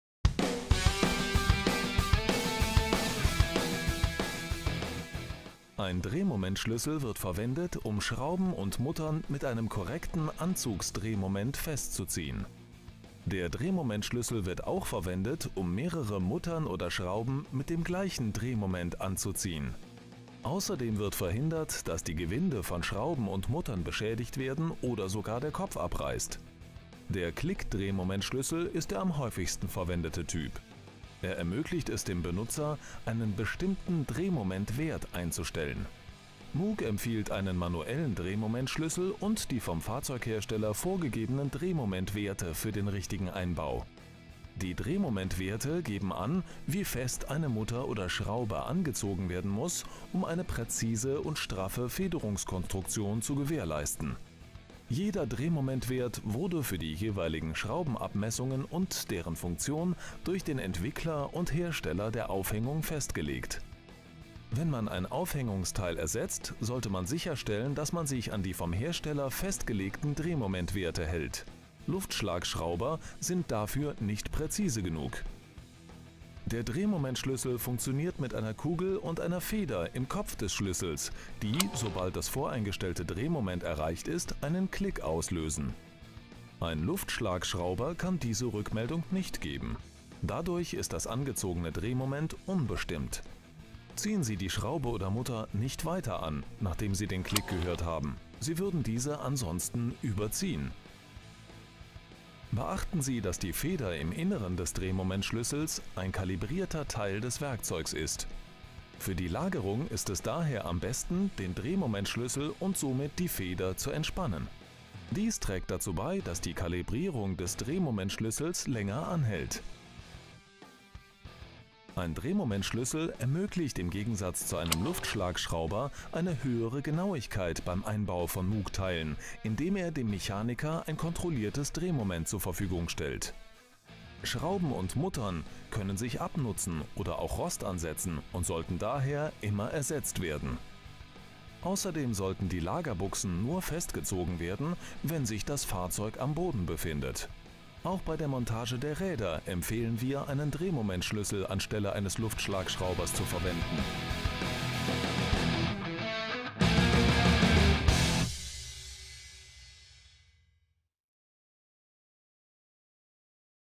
Wie man Muttern und Schrauben mit einem Drehmomentschlüssel anzieht
Ein Drehmomentschlüssel wird verwendet, um Muttern und Schrauben mit einem bestimmten Drehmoment festzuziehen. Es wird auch verwendet, um die gleiche Menge an Drehmoment auf mehrere Muttern und Bolzen anzuwenden, da darüber hinaus verhindert, dass das Gewinde der Muttern und Schrauben beschädigt werden oder sogar die Köpfe abschnappen.